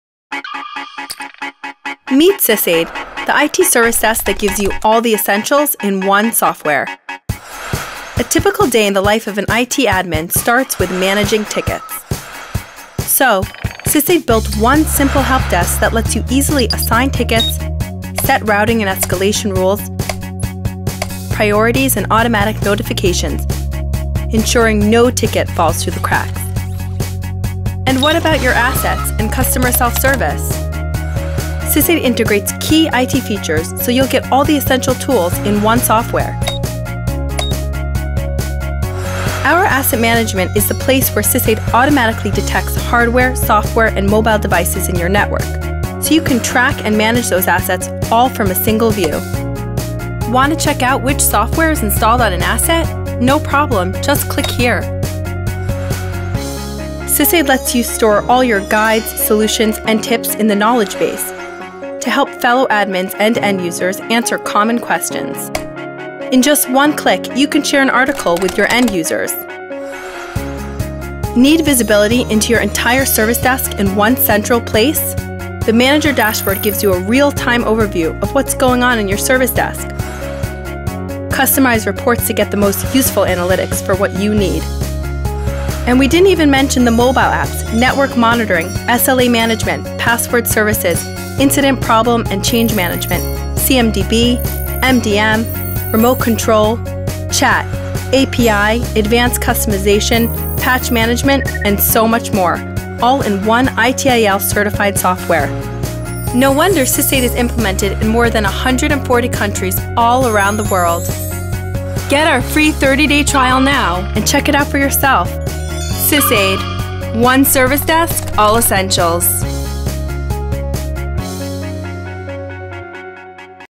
Conozca SysAid
Conozca SysAid, la Mesa de Ayuda para TI con todo lo esencial en un software.